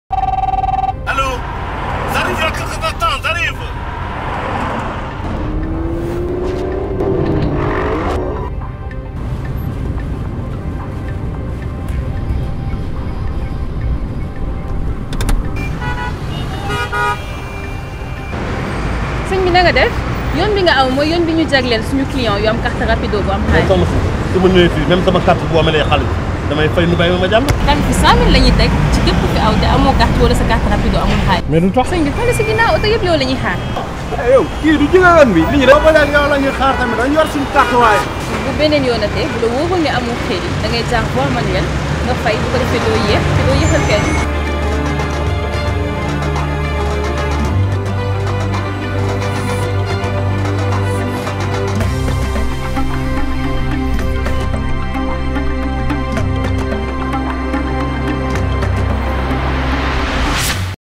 Passage abusif au niveau des voies de péage
Nous rappelons à l’ensemble de nos usagers que tout automobiliste qui tentera d'utiliser les voies RAPIDO sans badge ou sans crédit suffisant se verra appliqué un tarif unique de 5 000 Fcfa. Les voies automatiques RAPIDO sont exclusivement dédiées aux porteurs de cartes RAPIDO ayant un crédit suffisant.

Pensez à utiliser nos différentes solutions partenaires (Wari, Orangemoney, WizallMoney, JoniJoni, Tigocash, Yup, Postecash) pour consulter le solde de votre compte avant de vous engager sur l’Autoroute !